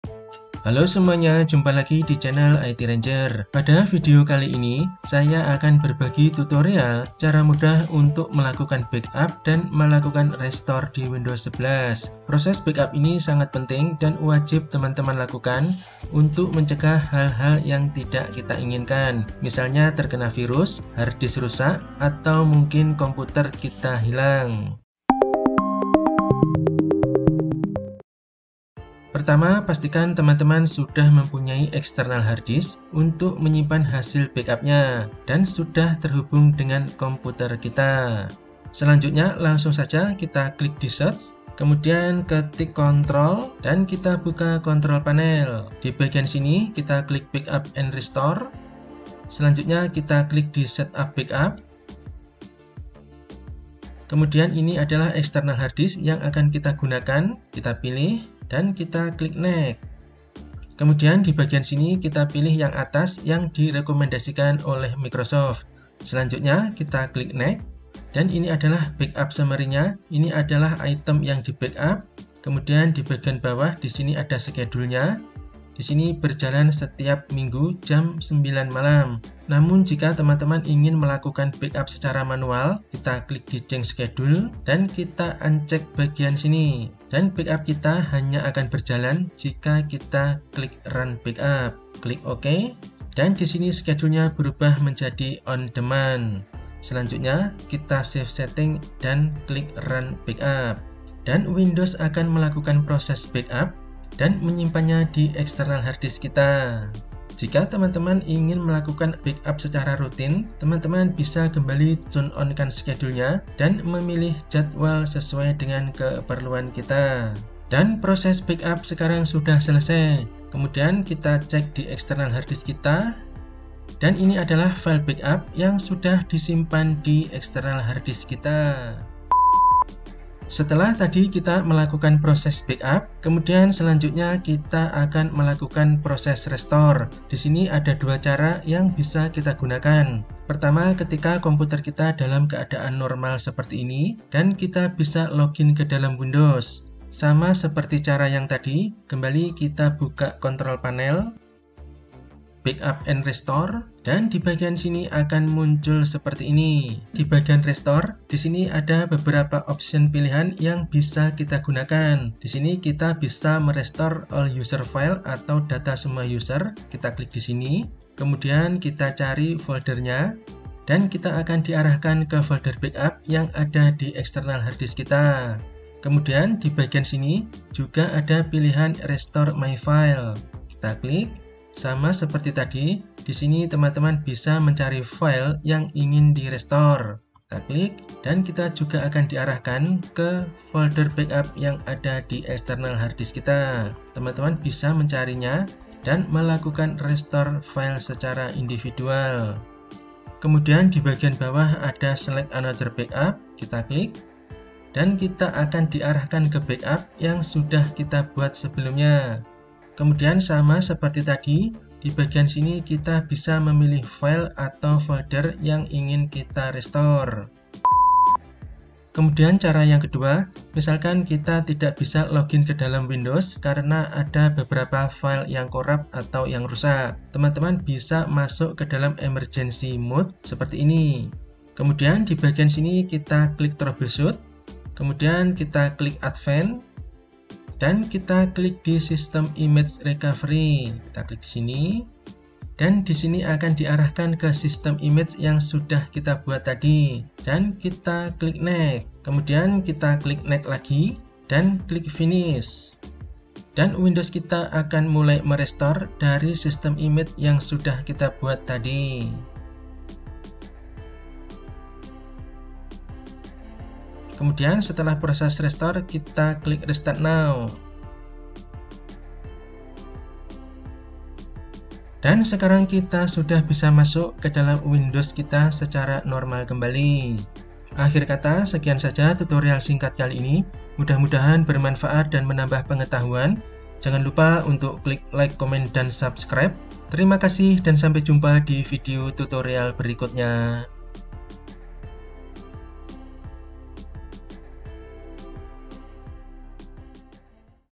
Cara Membuat Backup Image & Cara Restore Di Windows 11.
Backup/cadang adalah proses membuat data cadangan dengan cara menyalin atau membuat arsip data komputer sehingga data tersebut dapat digunakan kembali apabila terjadi kerusakan atau kehilangan. Backup memiliki dua tujuan.
Restore adalah tindakan untuk memulihkan sesuatu kembali seperti semula.